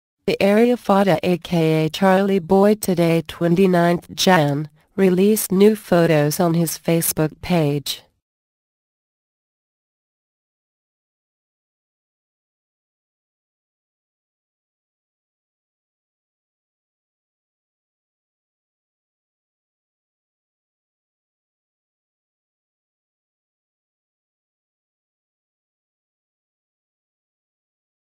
PHOTOS Charly Boy Releases New Photo
The Area Fada aka Charly boy today 29th Jan, released new photos on his Facebook page.